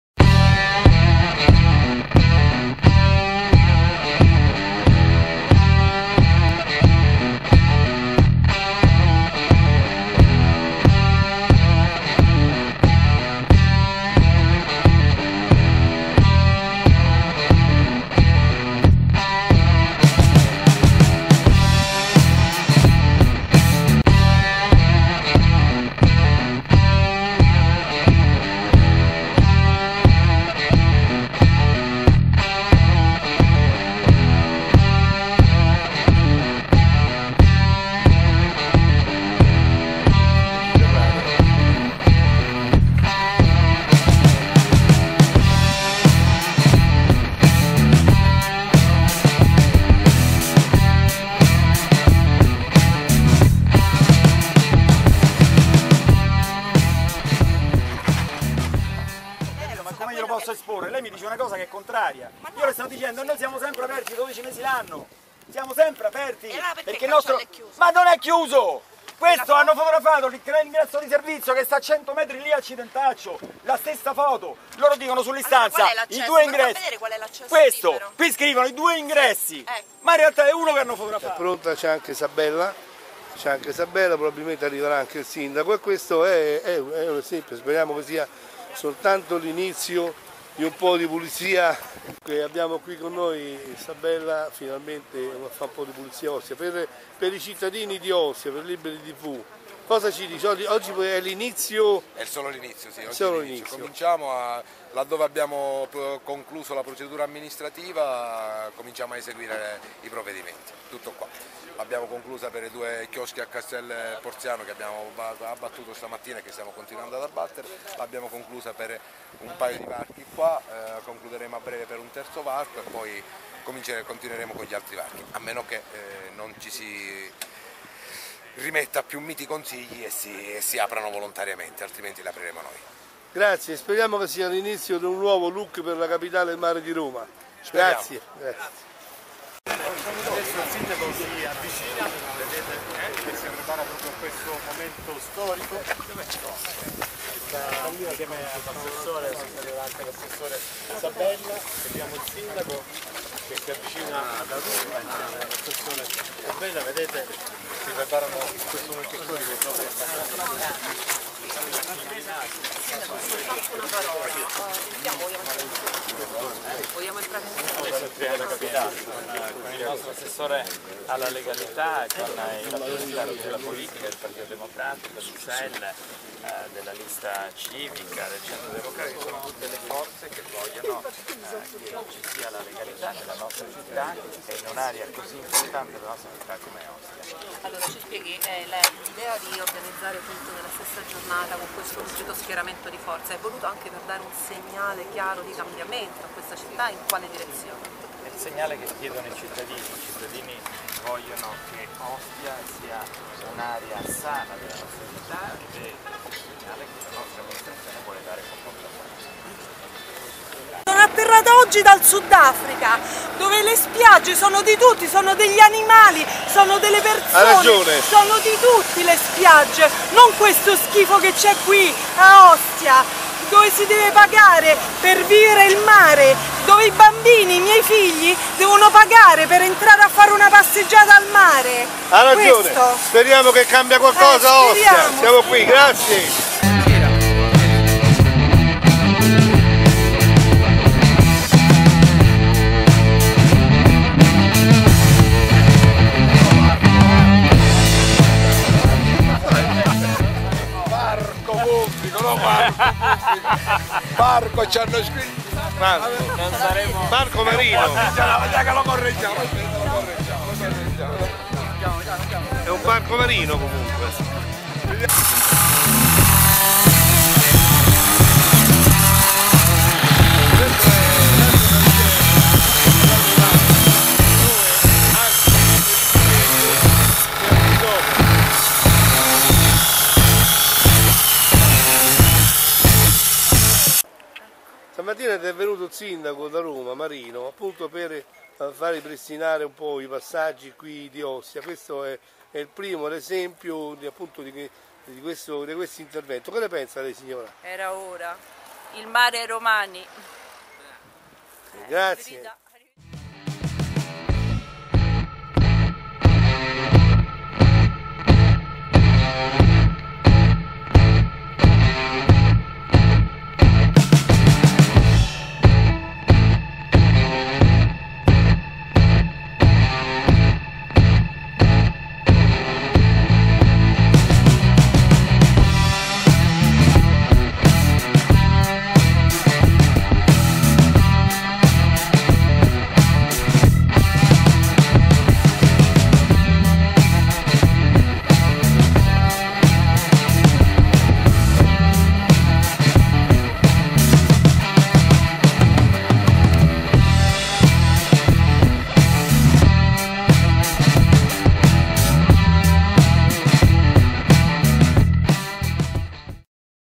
Ostia: La legalità arriva su ruote cingolate
Registrazioni effettuate da liberi.tv il 14/04/2015 a Ostia (Roma)
Primi abbattimenti di manufatti abusivi e aperture varchi di accesso alla spiaggia ripresi da Franco Giacomelli con intervista in esclusiva per Liberi.tv ad Alfonso Sabella, Assessore alla Legalità di Roma Capitale.